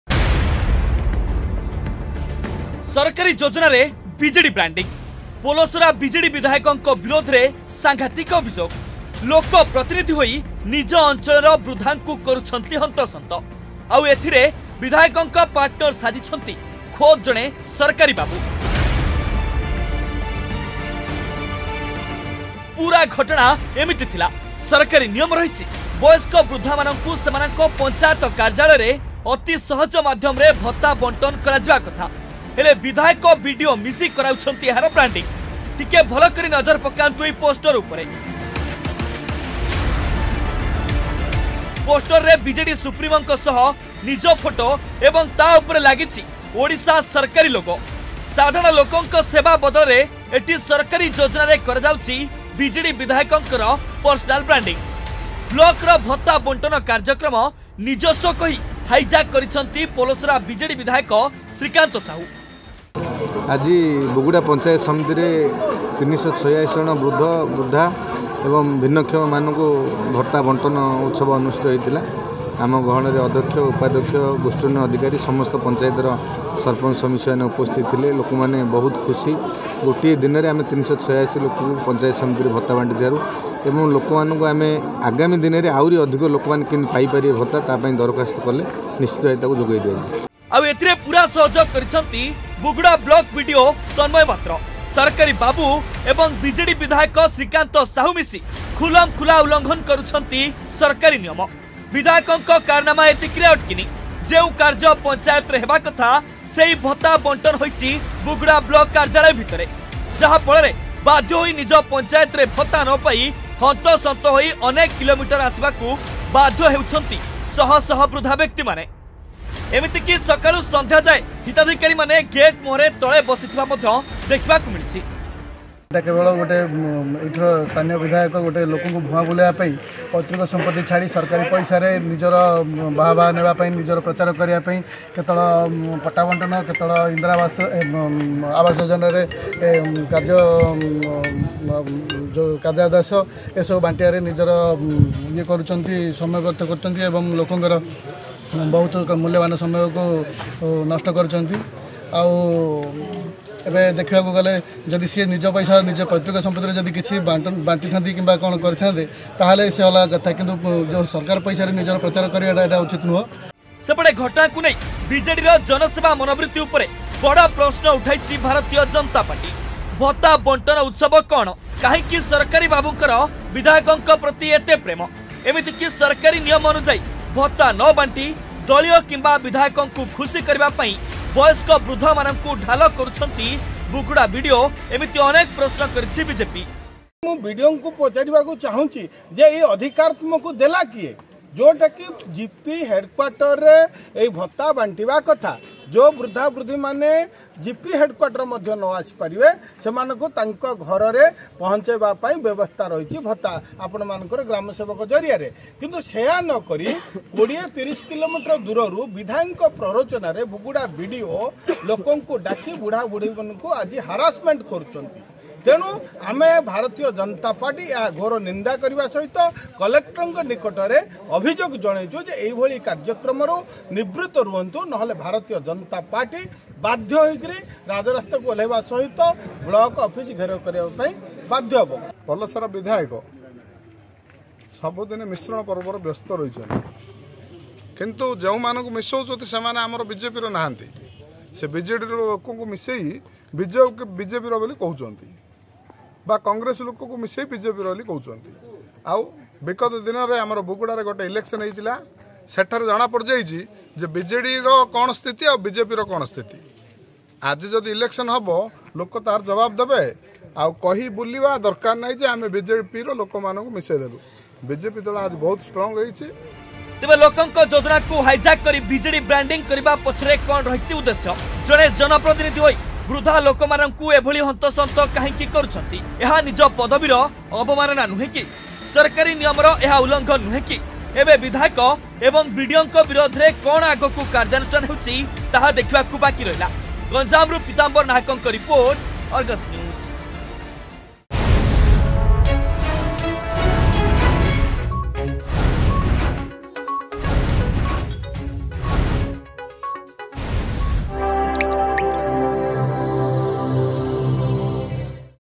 ସରକାରୀ ଯୋଜନାରେ ବିଜେଡି ବ୍ରାଣ୍ଡିଂ
ସରକାରୀ ଯୋଜନାରେ ବିଜେଡି ବ୍ରାଣ୍ଡିଂ  । ବିବାଦରେ ପୋଲସରା ନିର୍ବାଚନ ମଣ୍ଡଳୀର ବିଜେଡି ବିଧାୟକ ଶ୍ରୀକାନ୍ତ ସାହୁ ଏବଂ ବୁଗୁଡା ବିଡିଓ ତନ୍ମୟ ପାତ୍ର । ପଦାରେ ପଡିଲା ବିଡିଓଙ୍କ ବିଜେଡ଼ି ମୋହ ଏବଂ ଧରା ପଡିଲା କିଭଳି ଜଣେ ଜନପ୍ରତିନିଧି ବିଜେଡ଼ି ବିଧାୟକ ସହ  ବୃଦ୍ଧାଙ୍କୁ କରୁଛନ୍ତି ହନ୍ତସନ୍ତ ...
ଅଧିକ ଖବର ପାଇଁ ଡ଼ାଉନଲୋଡ କରନ୍ତୁ “Argus News” ଆପ୍